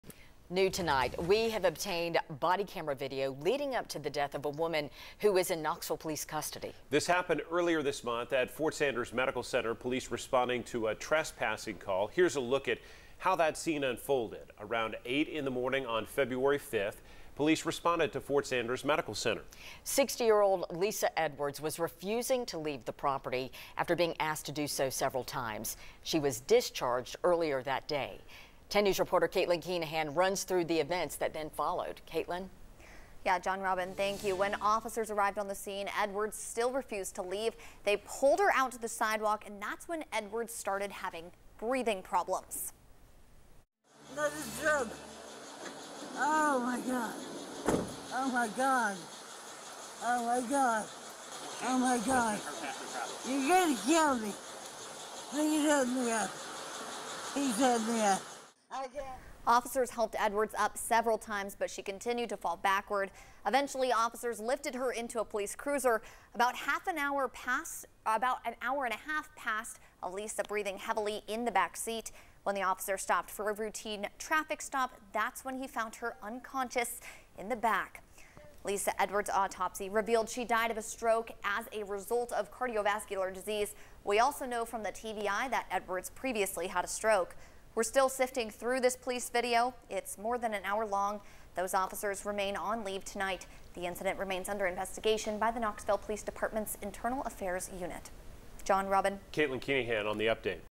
KPD releases body camera video of Lisa Edwards' death
Knoxville Police released body camera video leading up to the death of Lisa Edwards who was in police custody in early February.